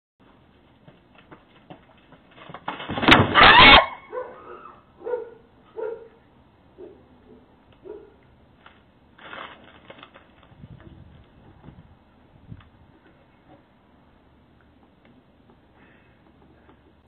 Ring Doorbell Catches Trip on Stairs || ViralHog
Occurred on November 27, 2021

A Ring doorbell camera captures a person tripping and falling while walking up steps to the front door.